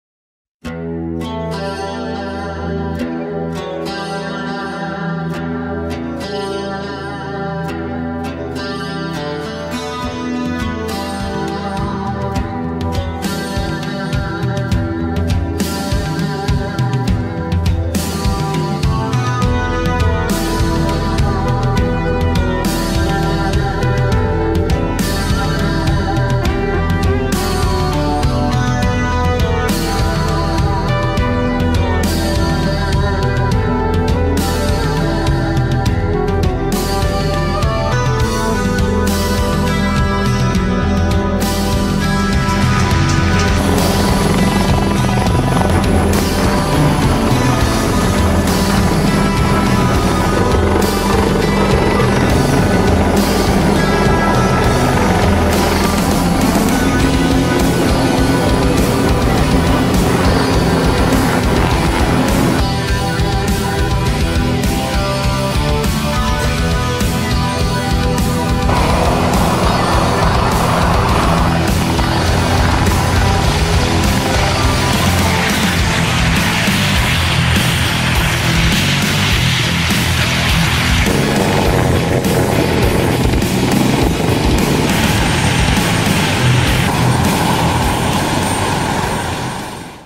H-225M Caracal Efektif Digunakan Satbravo 90 Kopasgat
Untuk memelihara kemampuan personel maupun alutsista, Satbravo 90 Kopasgat TNI AU melaksanakan latihan Fast roping dan simulasi penyelamatan sandera, menggunakan Helikopter H-225M Caracal.

Helikopter H-225M Caracal yang belum lama ini diserahkan Menhan Prabowo @prabowo kepada TNI AU, sangat efektif digunakan untuk mobilisasi udara, yang dapat secara cepat mengangkut pasukan menuju daerah sasaran.